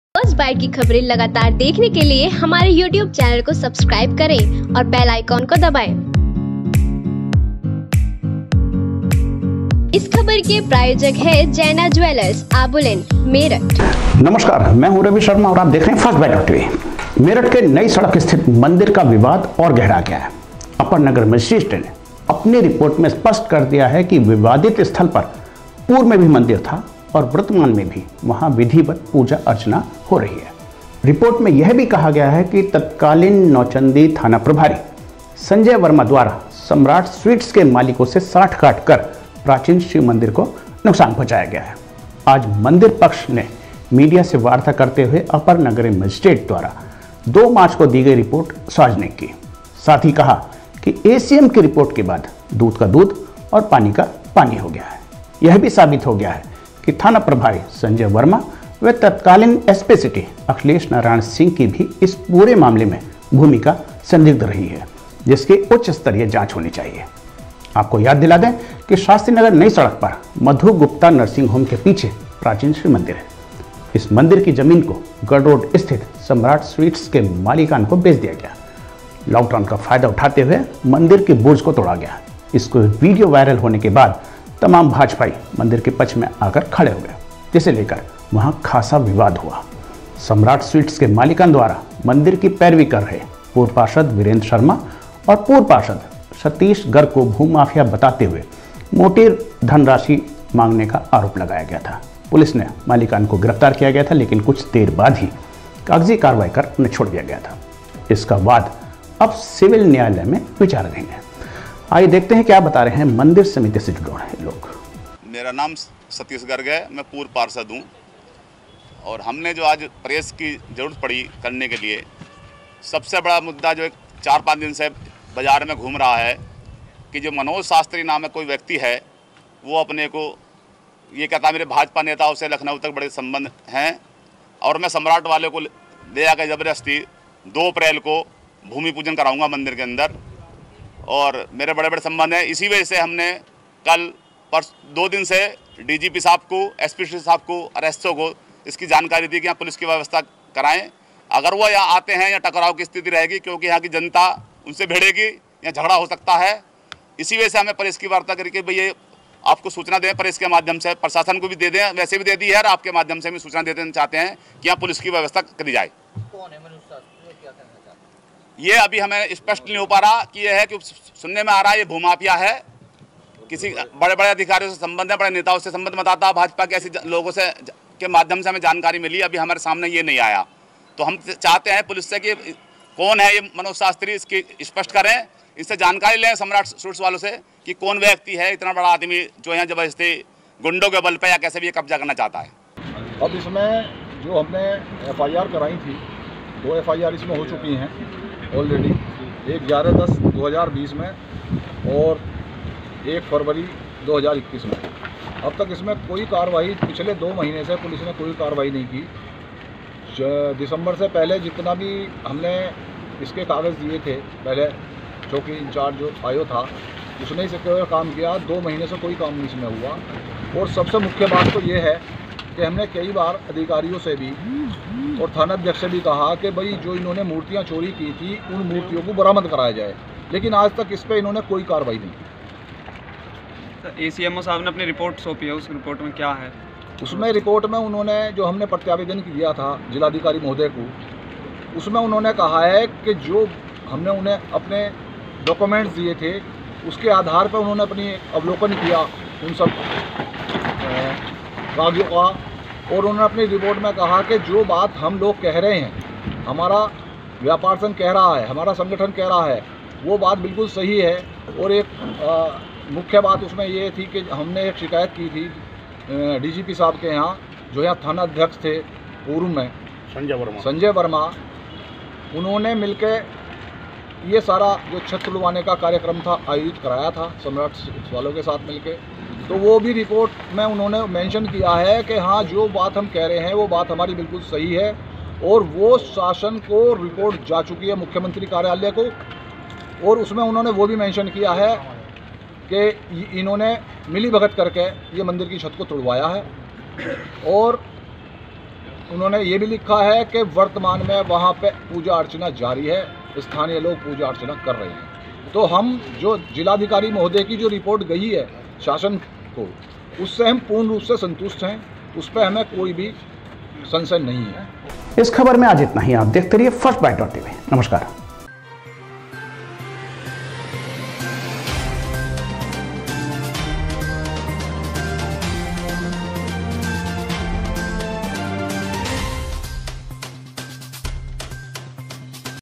नई सड़क मंदिर विवाद-एसीएम ने रिपोर्ट में नौचंदी थाना प्रभारी को दोषी बताया, मंदिर होने की पुष्टि
एसीएम ने अपनी रिपोर्ट में नौचंदी थाना प्रभारी को दोषी बताया ।।